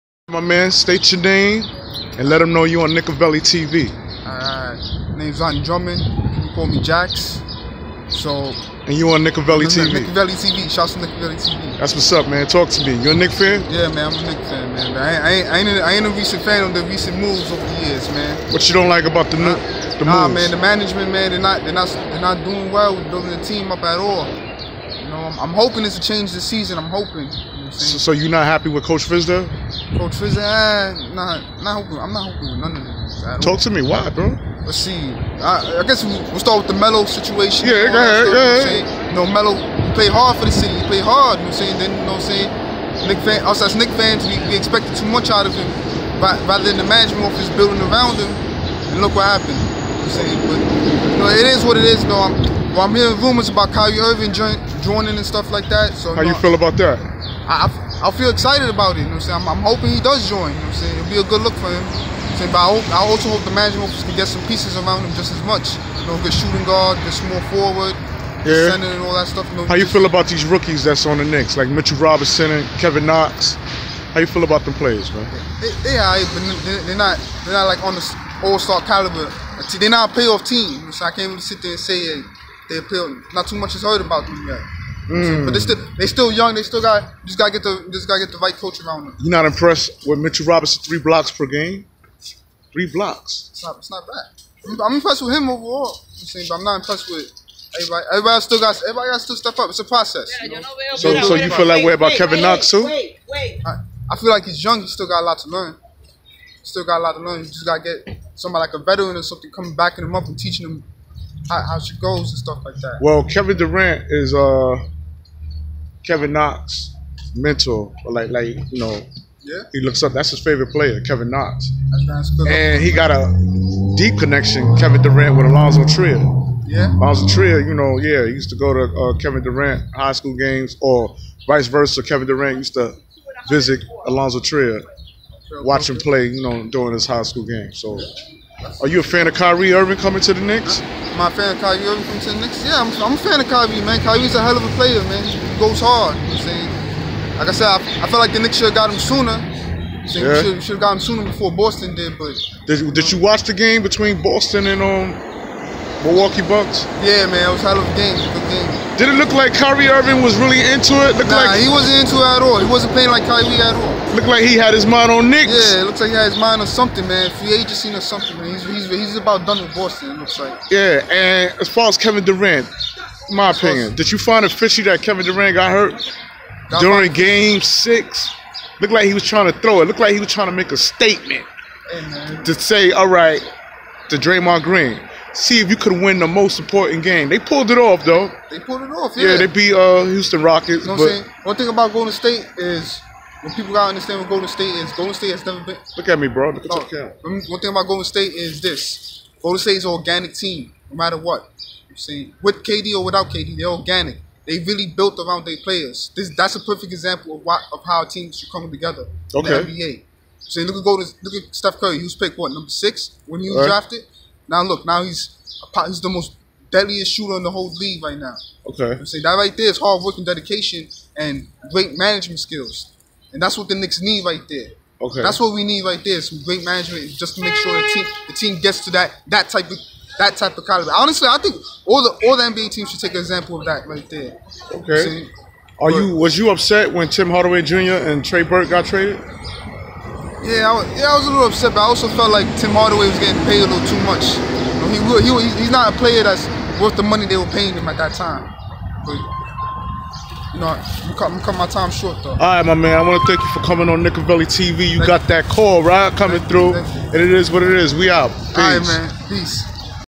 Mitchell Robinson is the only player that impresses me.🏀/ knick management did carmelo wrong.🏀
Prayers needed concerning knick management?🏀